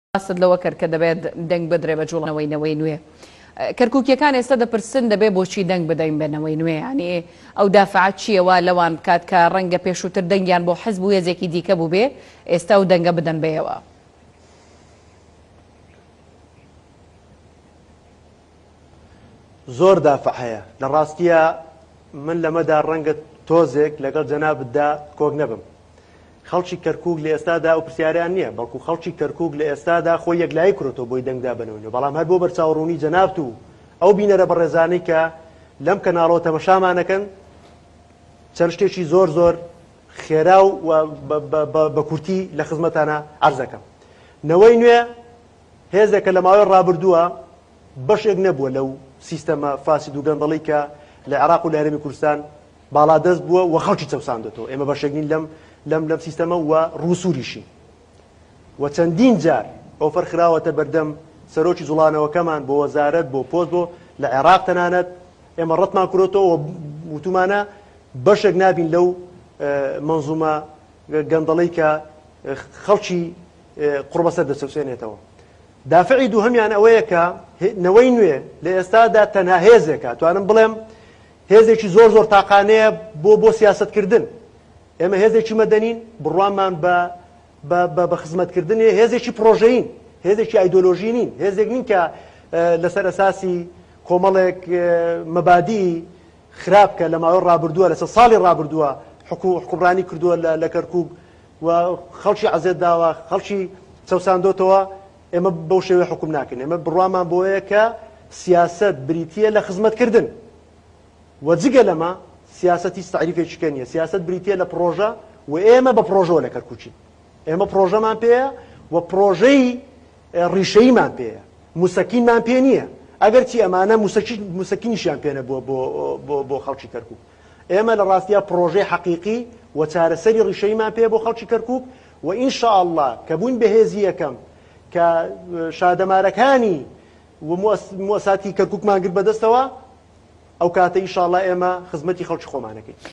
لێی دەپرسێت: بۆچی خەڵکی کەرکوک دەنگ بدەن بەنەوەی نوێسەرۆکی لیستی نەوەی نوێ وەڵام دەداتەوە
لێی دەپرسێت: بۆچی خەڵکی کەرکوک دەنگ بدەن بەنەوەی نوێ
سەرۆکی لیستی نەوەی نوێ وەڵام دەداتەوە